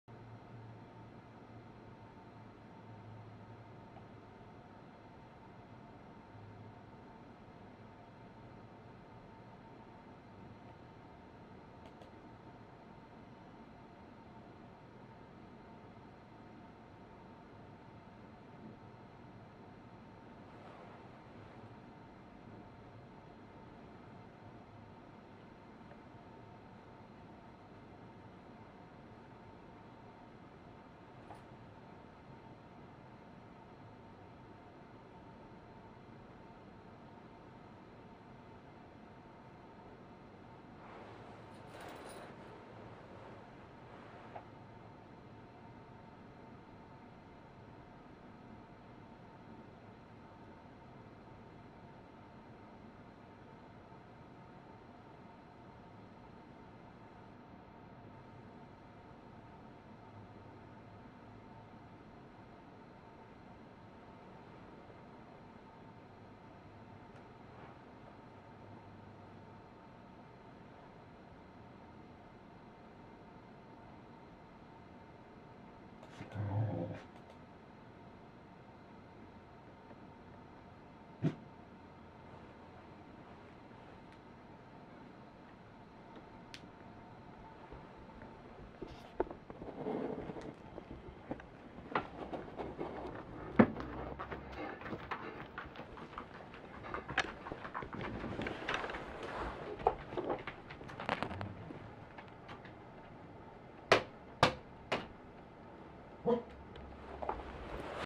Goro is sleepy watch dog. / おねむだけど番犬のお仕事をするゴローさん 20150213 Goro@Welsh corgi コーギー
眠いので静かに吼えてますw